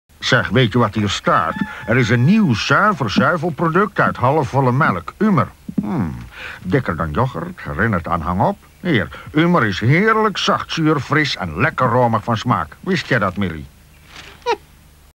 TV: Umer - Nieuw Zuivelproduct (1987) reclame
Reclamespot van Umer uit 1987.
Met dank aan Guido voor het beschikbaar stellen van deze videoband.
Source: Betamax.